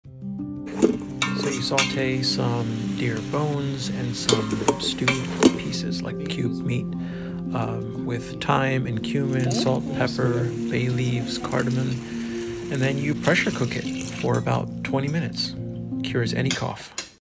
Easy Venison bone Soup for cough
My daughter came back from school with one of those nasty winter coughs 😷 I pulled out my bag of deer bones from the freezer and made this quick soup (You can do the same with chicken or beef)
- bones
- a few cubed meat pieces
- 1 onion / shallots
- bay leaves
- cardamom seeds
- salt , pepper , cumin , thyme

Sauté bones and meat pieces in some butter of ghee, add all spices and onions to extract the flavors out before adding water…sear for about 5 min. Then Add water and pressure cook for 20 min. In a normal pot, it’ll take maybe 45min.

I serve with rice and a squeeze of lemon.
 So yummy on a chilly day 😋 🥣

Allah is the source of all healing and is Ash-Shafee (The Healer)